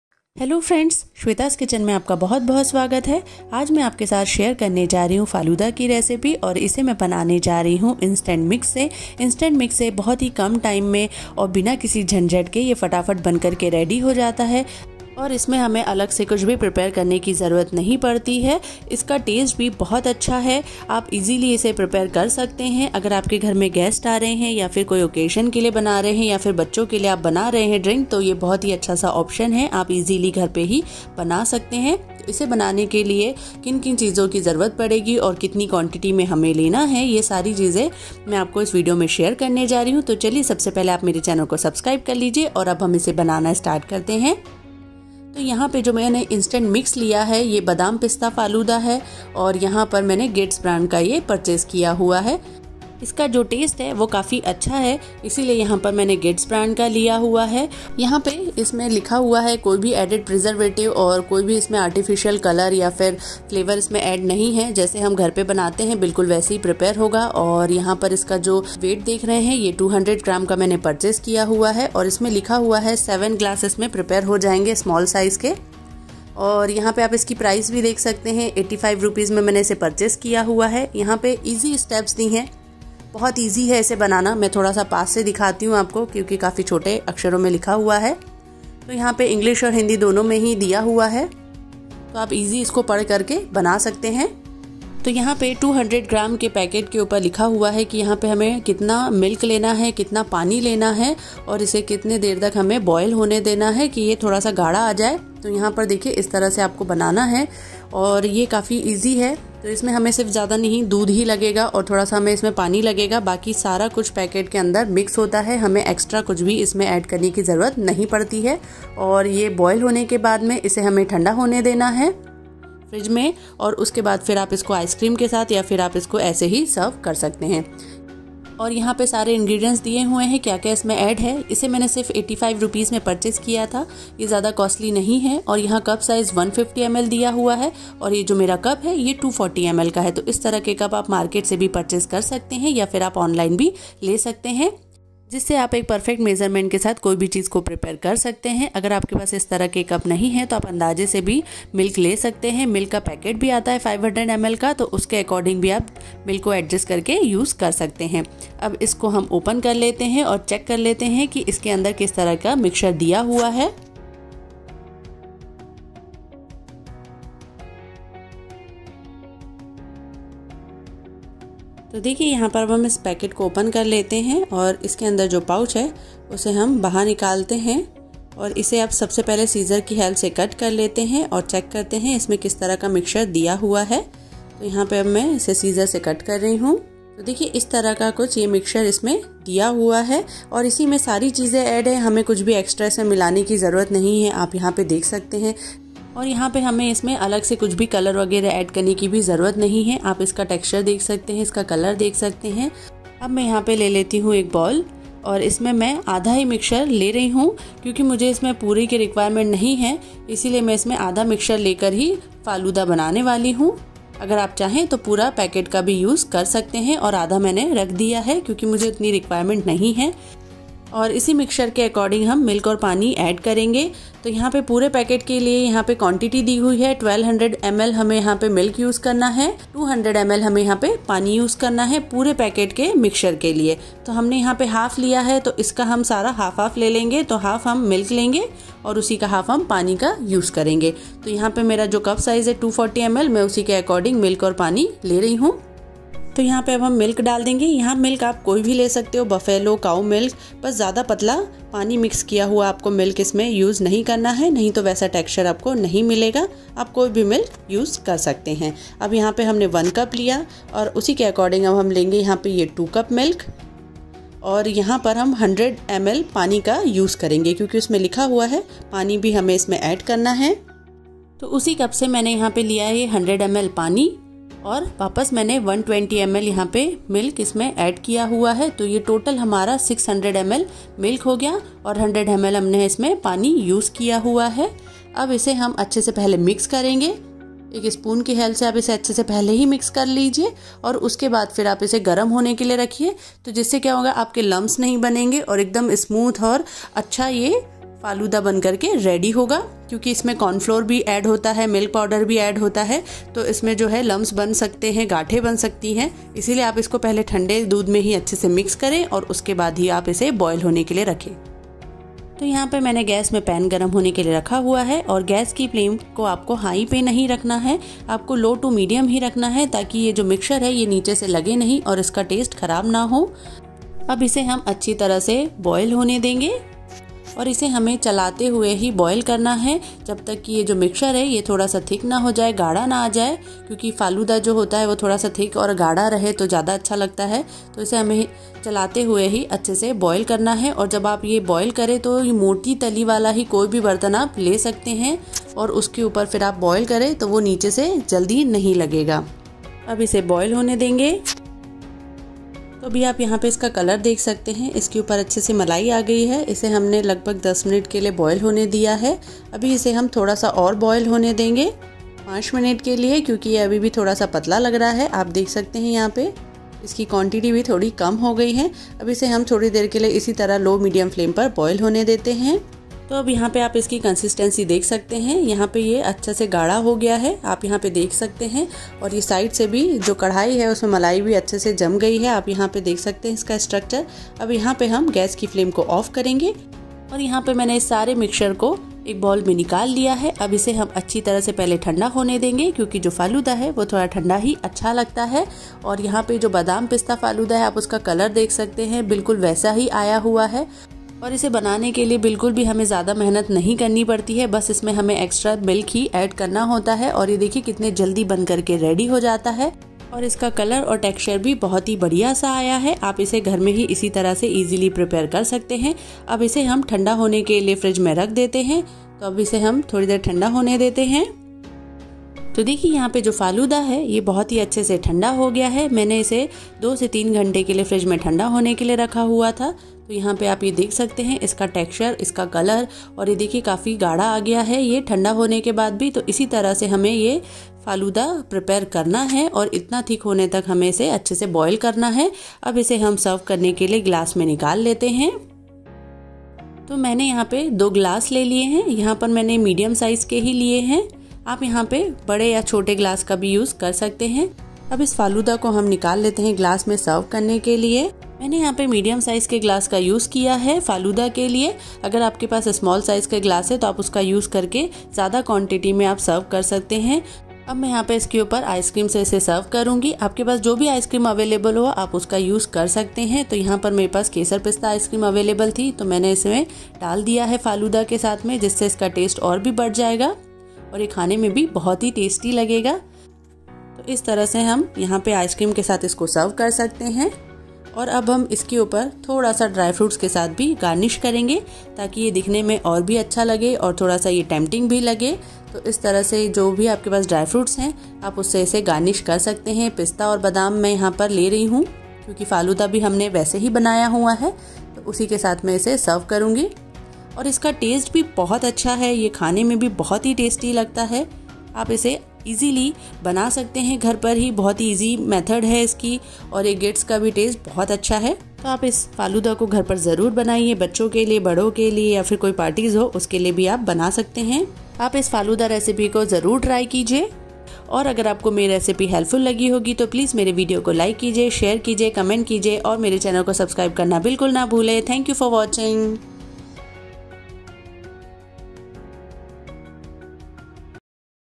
Gits Falooda recipe, Badam pista falooda, gits faluda recipe, Instant falooda, how to make falooda
Hello friends, today i brought a new recipe of Gits Badam pista falooda recipe with drink mix. Falooda recipe is very easy and simple. Please follow the steps shown in video to learn how to make falooda at home with gits drink mix. it is an easy falooda recipe.